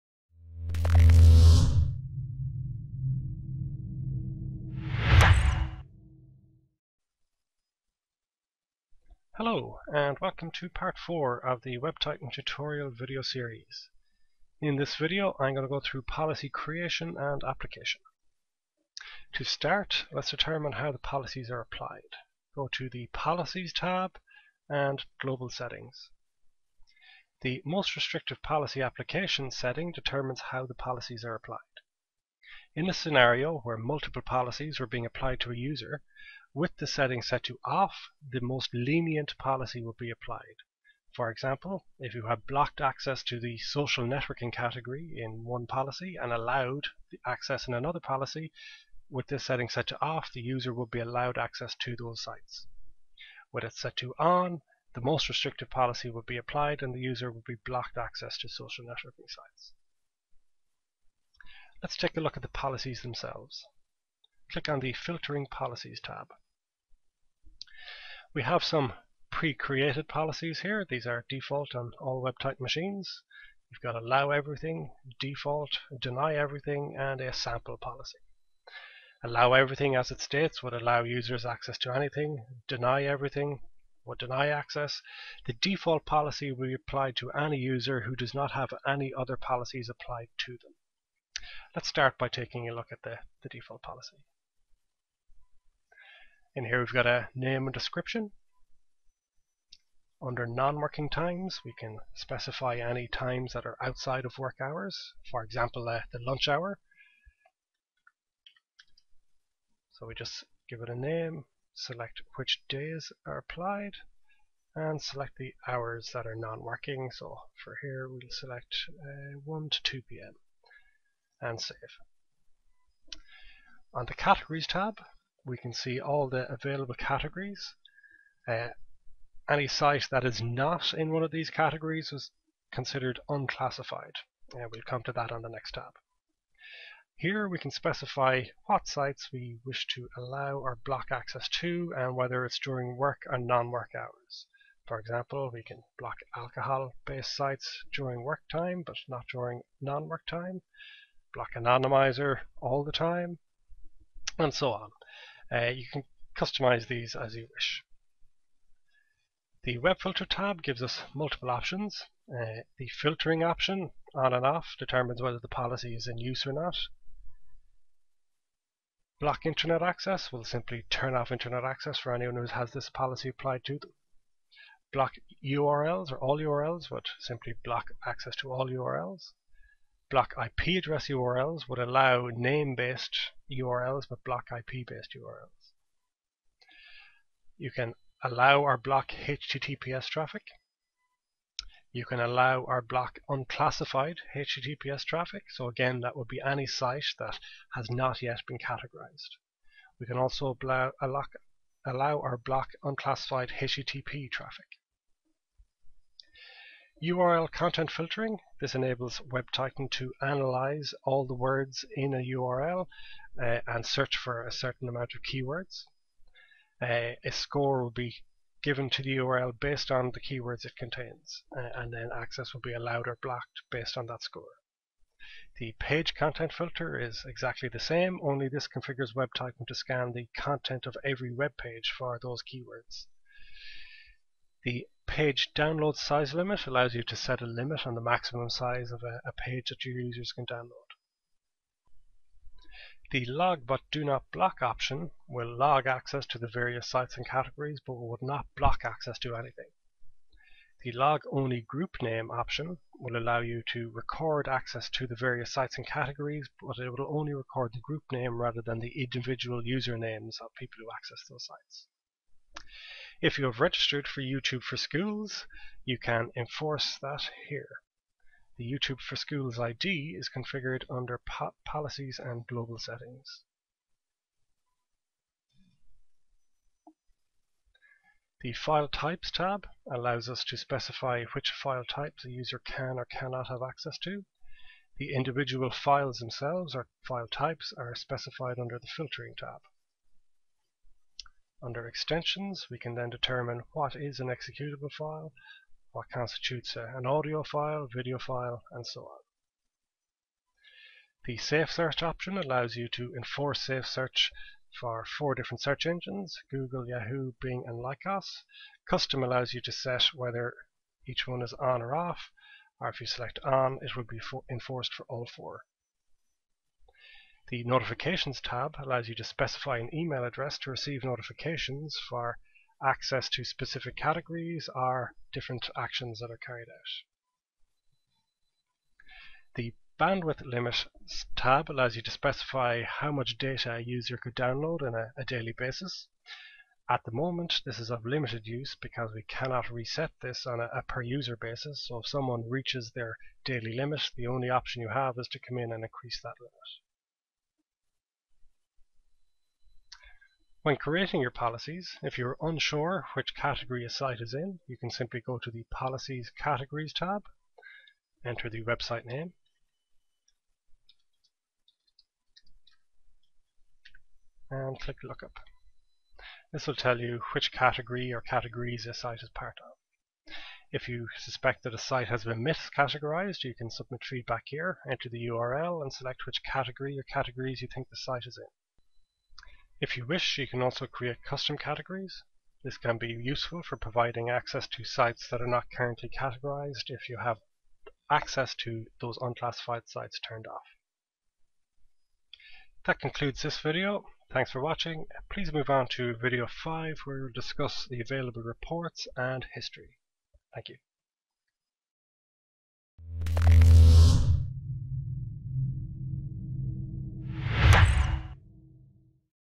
WebTitan 5.00 Set Up: Part 4 - Policies
This video outlines policy creation and application.